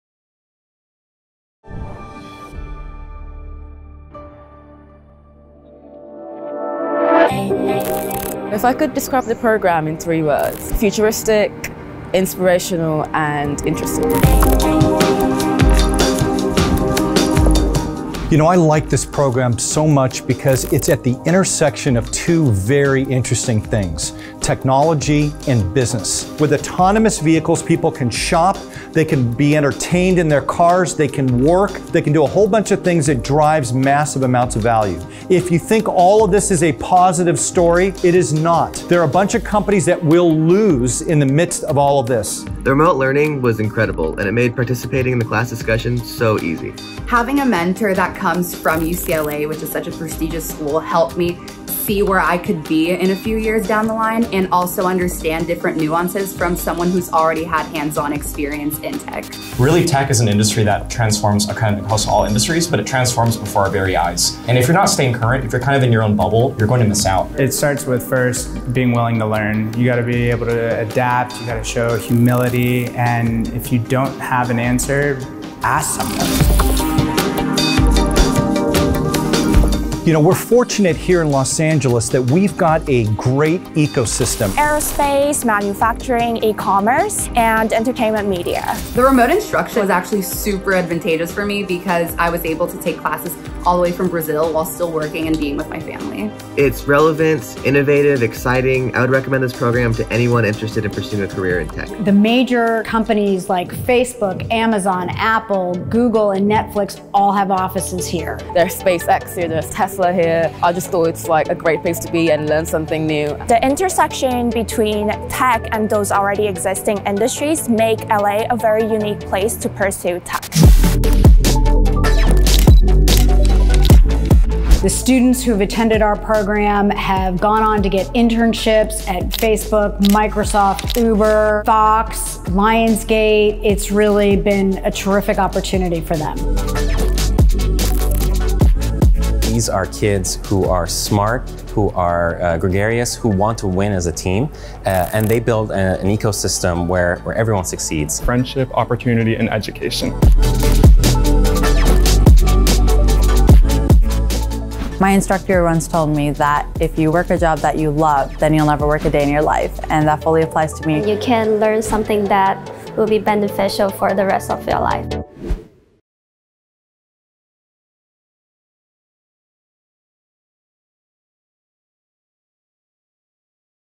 UCLA Technology Leadership Summer Institute
The UCLA Technology Leadership Summer Institute prepares undergraduates and young professionals for careers in tech management through classroom curriculum combined with special guest speakers and mentors who give students a look inside some of the most legendary high-tech companies. Derived directly from UCLA Anderson’s world-renowned MBA curriculum, the six-week program features top business school instructors and key leaders in today’s tech world.